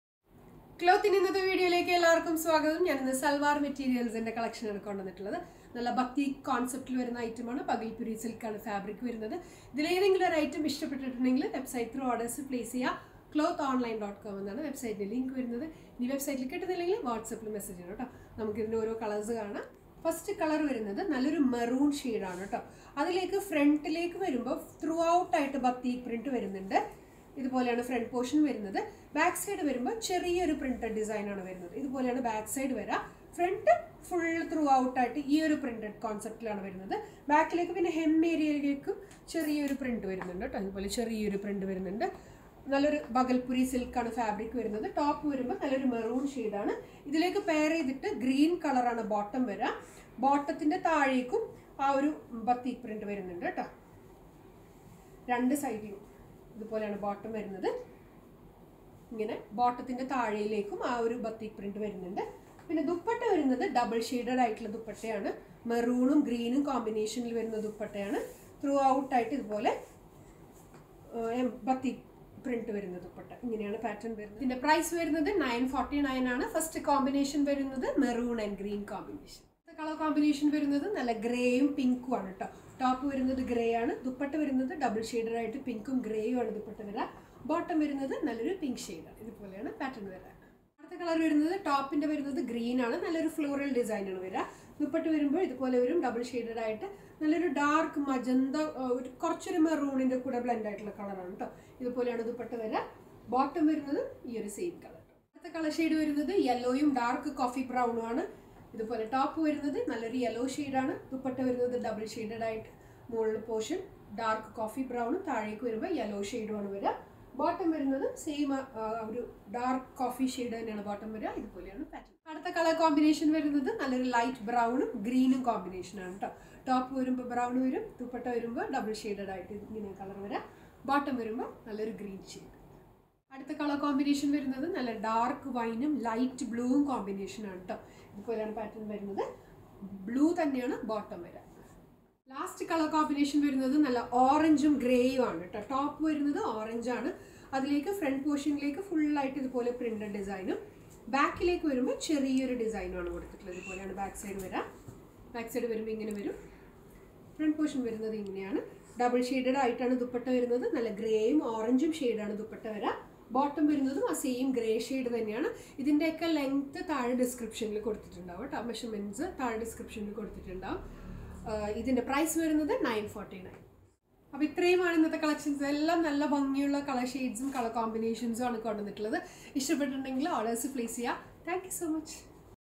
Salwar Materials ✨kloth_fashion_store
Salwar Materials ✨
Top material 2.60 metres
Bottom Material  2.00 metres
Dupatta 2.30 Metres
Price 949/-📌📌
Code no 1677
🔶No COD🔶

Shipping - Delivery in 5 -10 working days ( if not affected by any other situation )

Pls note :-
The colours you see on your device may slightly vary due to the difference in individual monitor

Thank you for reaching us 💕💕